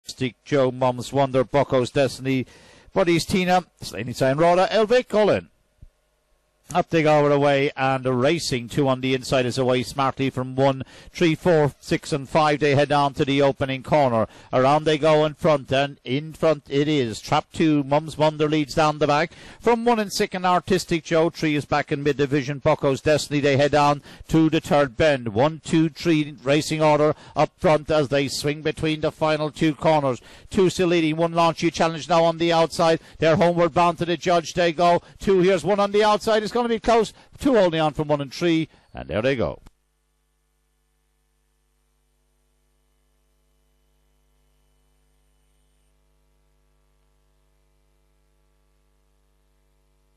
IGB - The Slan Abhaile 13/12/2018 Race 10 - Curraheen Park
Race: The Slan Abhaile
Track Name: Curraheen Park
Date: 13/12/2018
Hour: 22:05

Results

1st    2     MUMS WONDER         2/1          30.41
2nd    1     ARTISTIC JOE        6/4f         30.48
3rd    3     BUCKOS DESTINY      2/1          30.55
4th    4     BUDDYS TINA         4/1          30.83
5th    5     SLANEYSIDE RHONA    4/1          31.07
6th    6     EL VIC              3/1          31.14

|CRK_2018_12_13_10